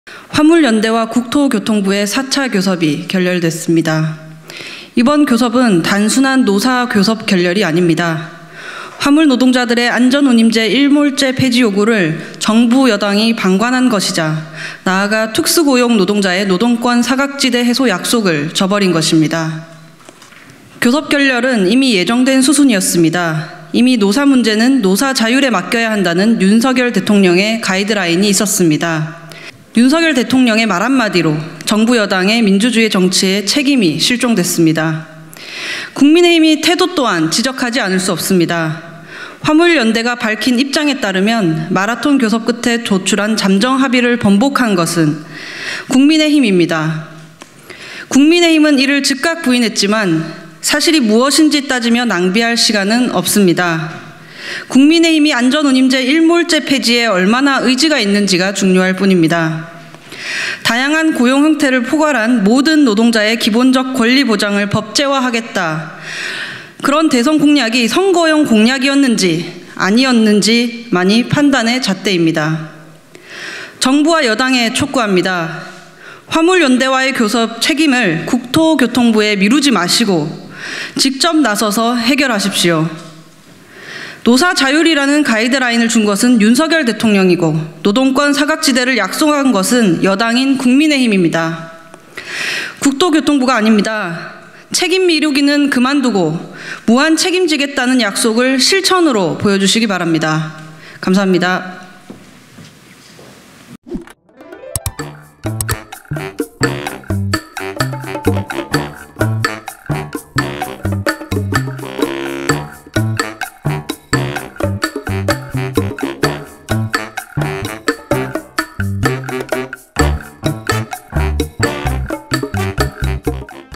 화물연대-국토부 4차 교섭 결렬... 류호정 "정부·여당의 민주주의 정치 책임 실종되었다"
정의랑 류호정 원내대변인이 22년 6월 13일 국회 소통관 기자회견장에서 화물연대와 국토교퉁부의 4차 교섭 결렬과 관련해 기자회견을 하고 있다.